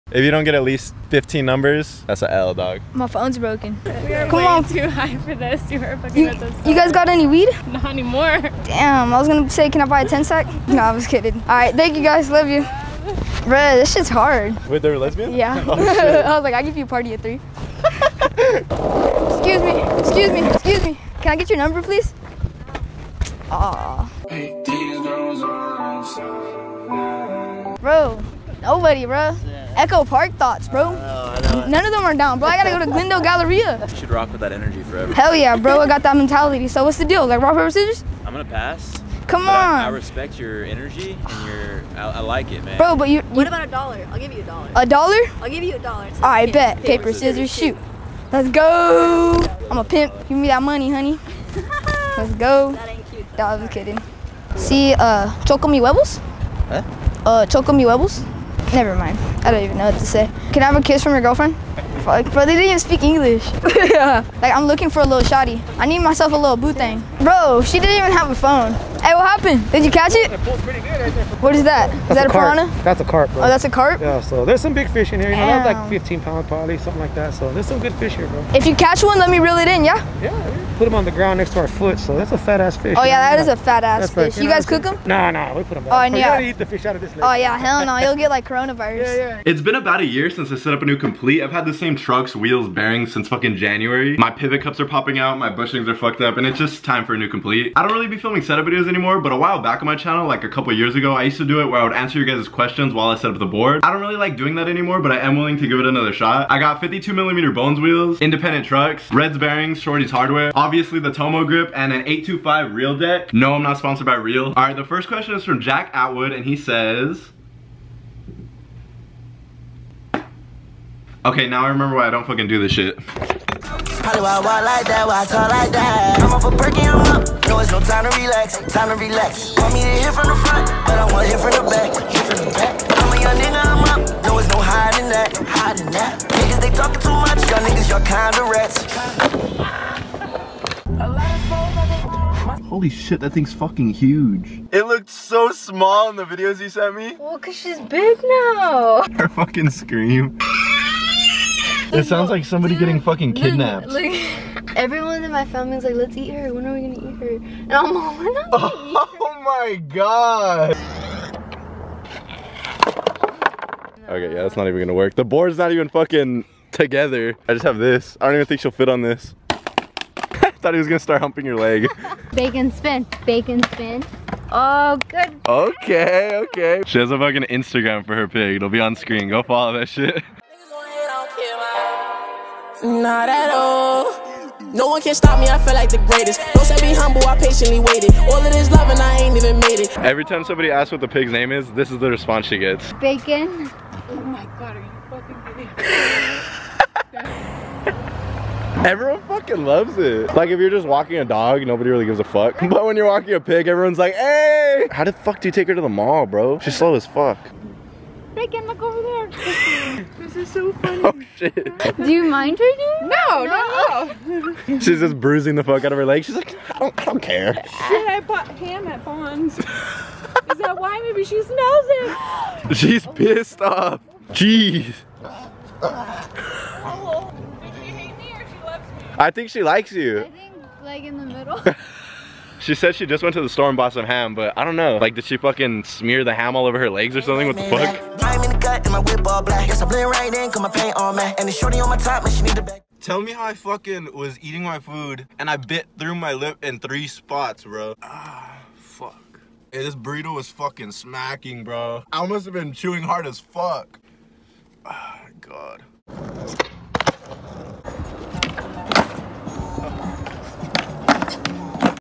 Pigs Are Underrated!
Who would've thought that a pig would be so entertaining to watch lmao. Thank you guys for all the support on this channel!